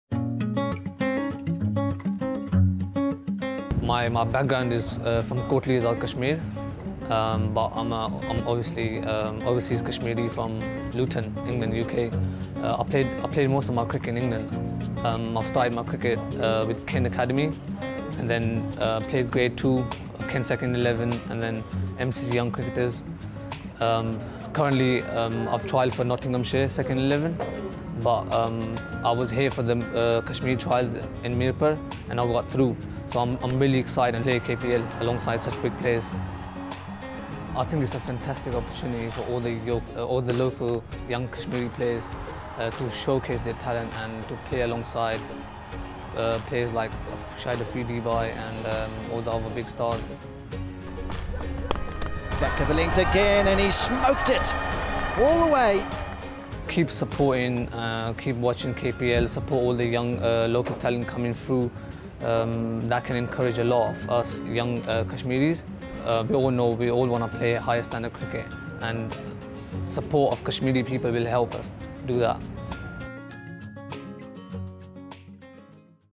KASHIF ALI | RAWALAKOT HAWKS | KPL
Overseas Kashmiri from Luton, UK Kashif Ali of Rawalakot Hawks Commending Kashmir Premier League. Aiming and Recommending to support KPL for a prosperous of Kashmir.